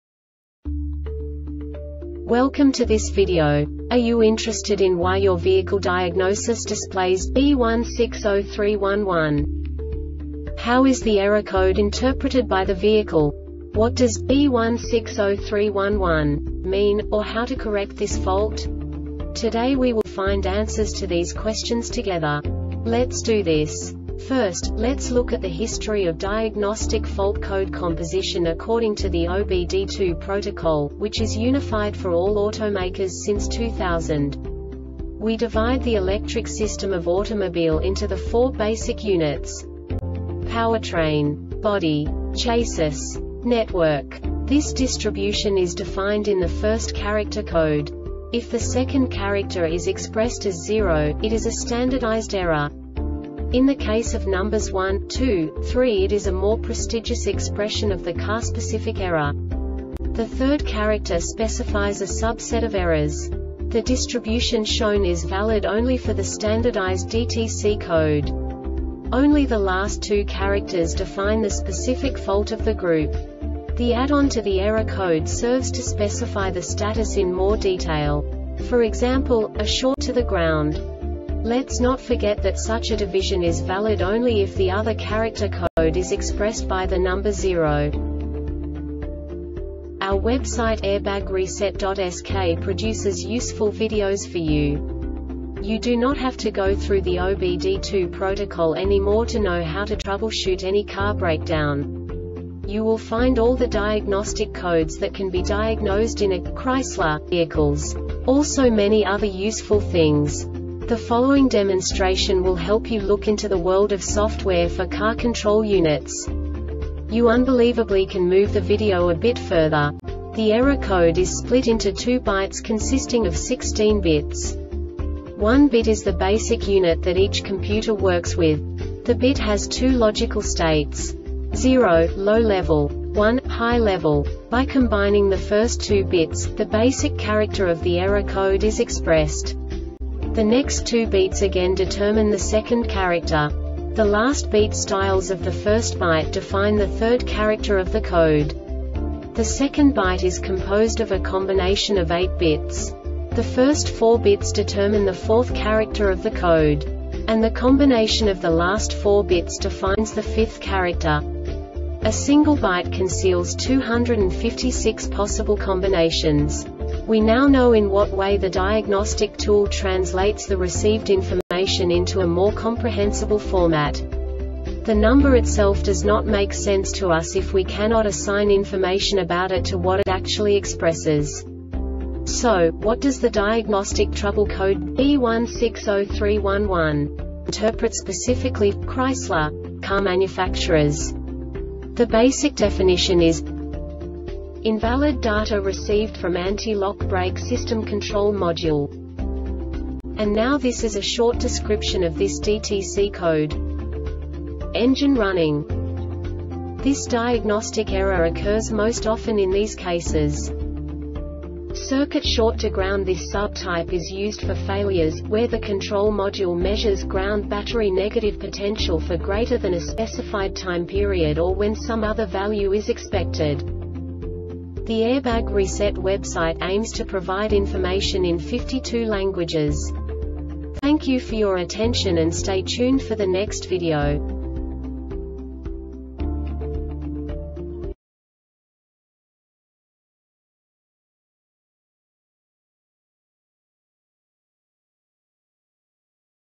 DTC Chrysler B1603-11 Short Explanation
Contents:
0:21 Basic DTC analysis according to OBD2 protocol standard.
1:48 Insight into programming
2:58 A diagnostically understandable description of the Diagnostic Trouble Code
3:05 Basic error definition

Make:
Chrysler
Code:
B1603 11
Definition:
Invalid Data Received From Anti-Lock Brake System Control Module
Description:
Engine running
Failure Type: